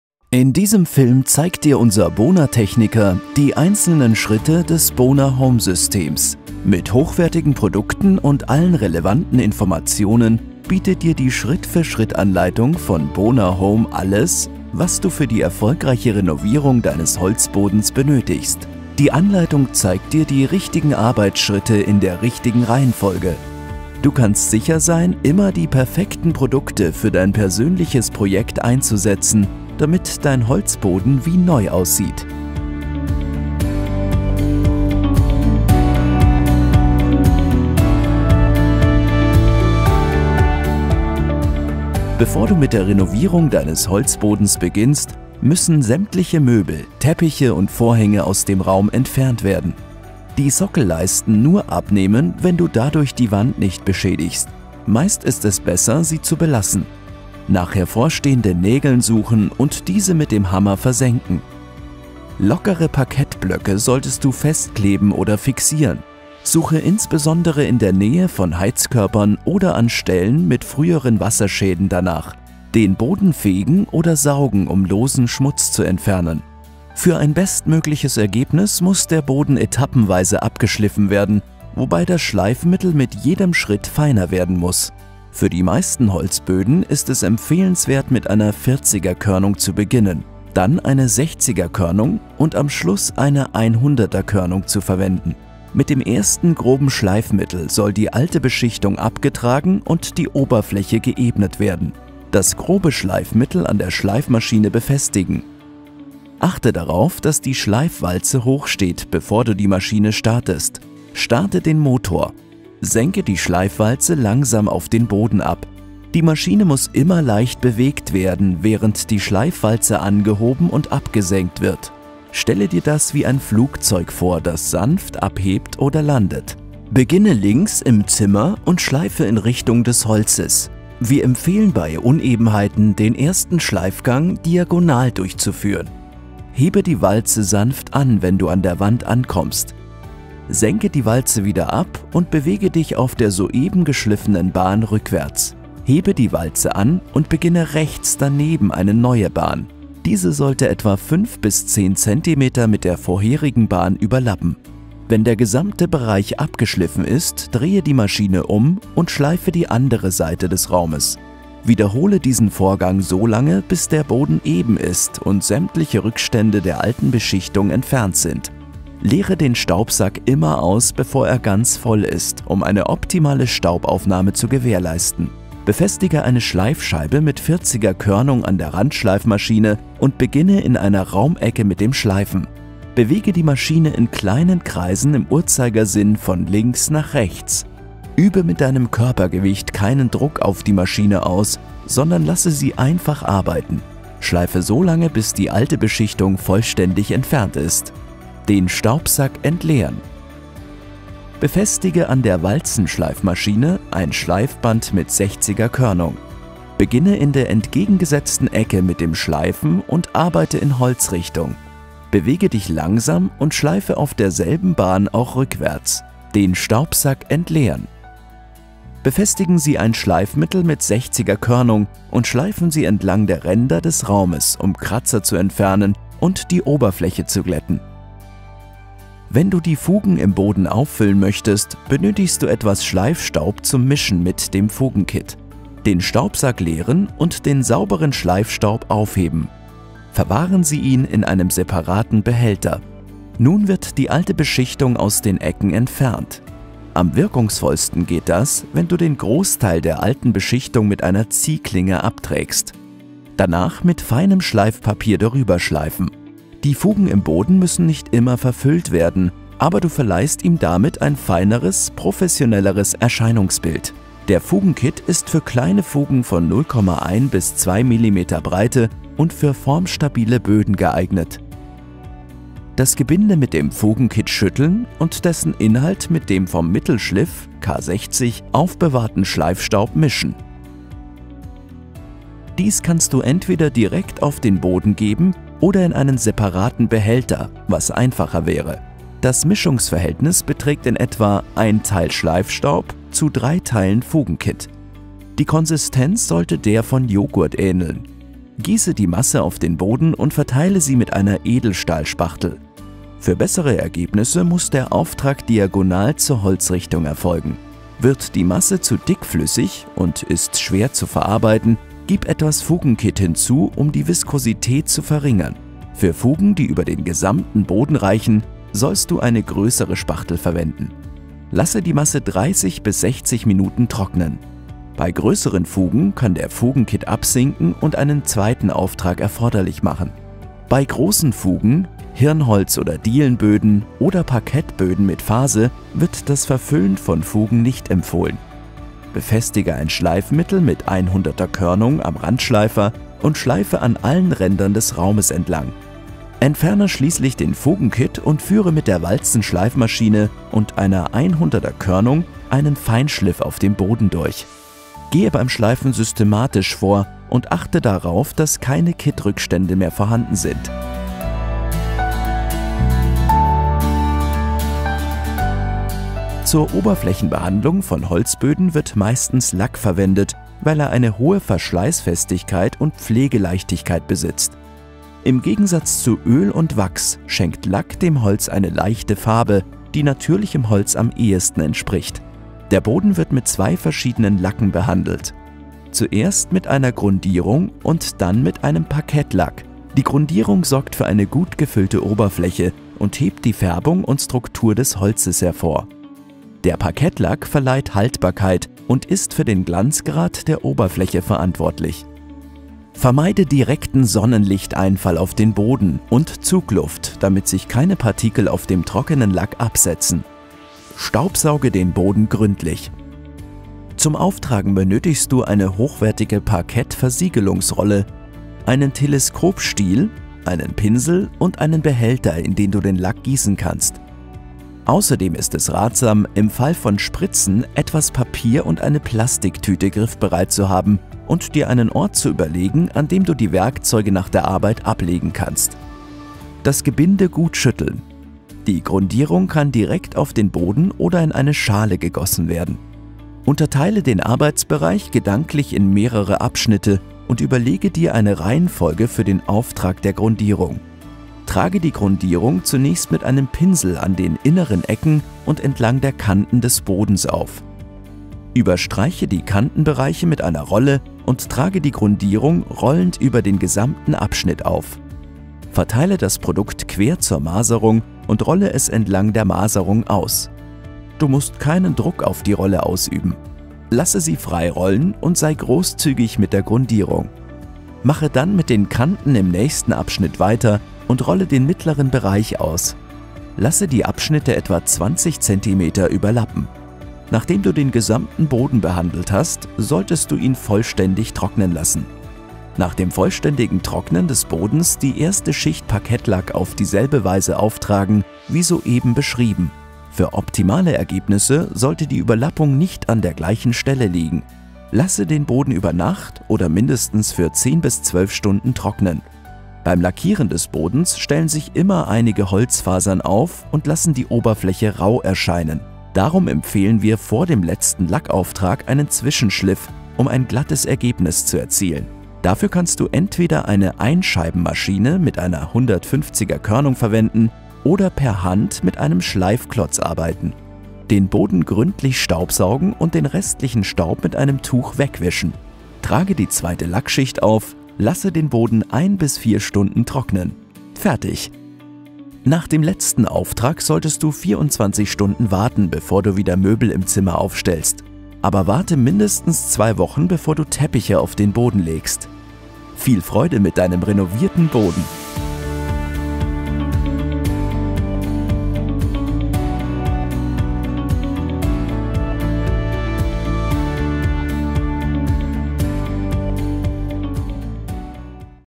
BONA - Parkett schleifen und mit BONA Home Parkettlack versiegeln. Eine Anleitung für Heimwerker.
Du willst deinen Holzböden renovieren? Bona Home macht's möglich. Wir zeigen dir Step-by-Step, wie es geht. Hier findest du eine genaue Anleitung zum Thema Parkett schleifen und mit Bona Home Parkettlack behandeln.

0:00 Vorbereitung
1:19 Schleifen
2:58 Grobschliff
3:24 Mittelschliff
4:21 Fugenkitten
5:47 Endschliff
6:02 Holzboden versiegeln

Weitere Anleitungen über Parkett ölen oder mit Bona Home Hartwachsöl behandeln findest du auch auf YouTube.